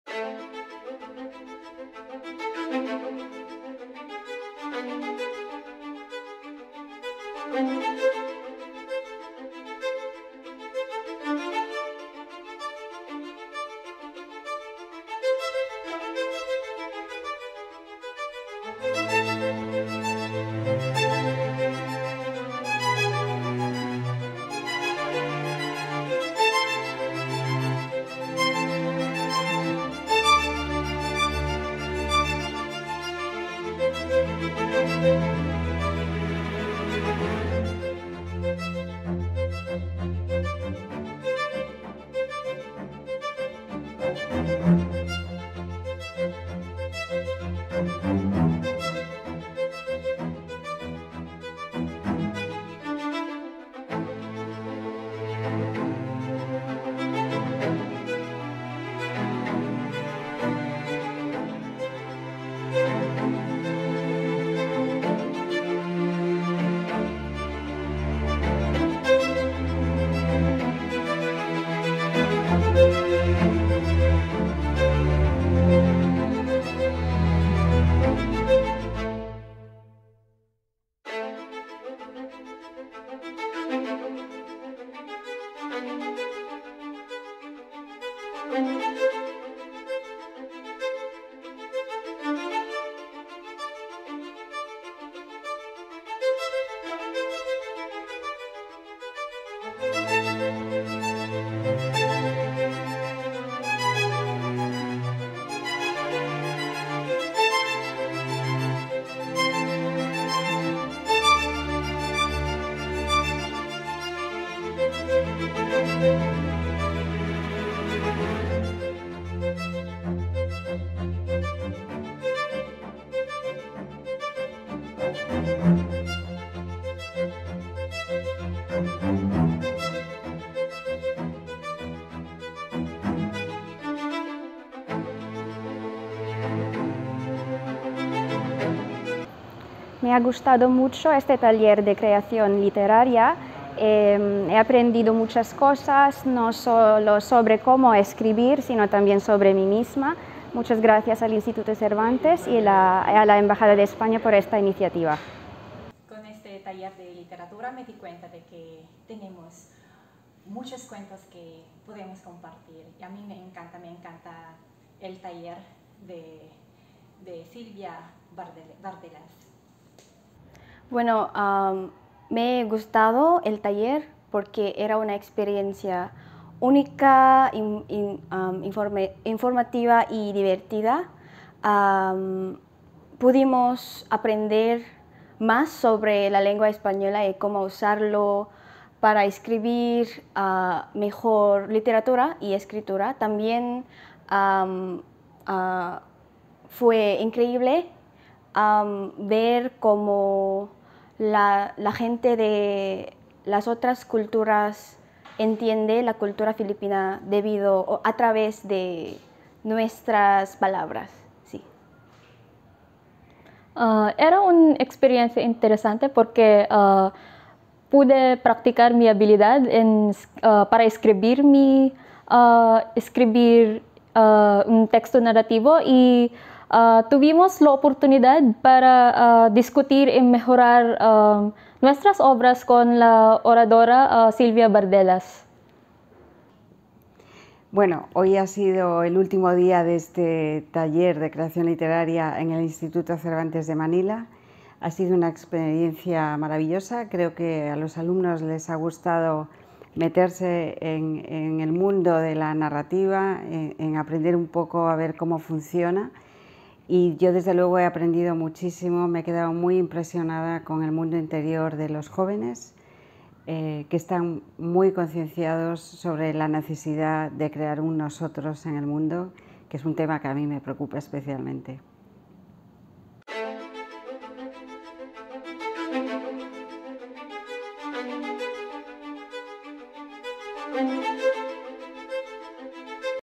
Taller de creación literaria con Silvia Bardelás (II) 13 de noviembre de 2022
Segunda jornada del taller de creación literaria con la escritora y editora Silvia Bardelás. Celebrado en el Instituto Cervantes de Manila en Intramuros los días 12 y 13 de noviembre de 2022.